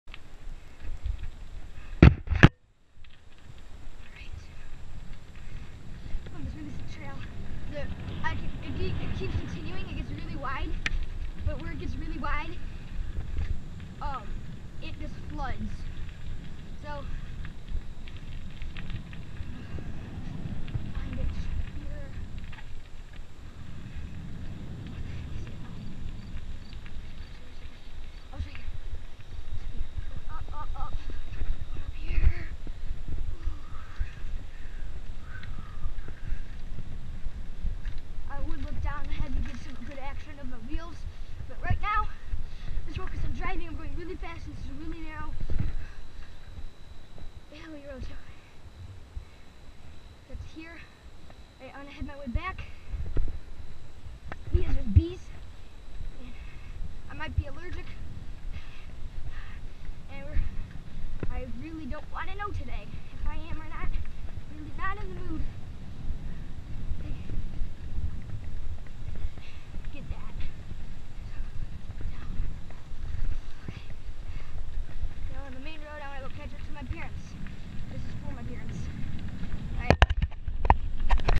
GoPro view going through bike trails on Belle Isle in Detroit
Bike ride through the trails on Belle Isle by the lighthouse viewed from wearing a GoPro on helmet.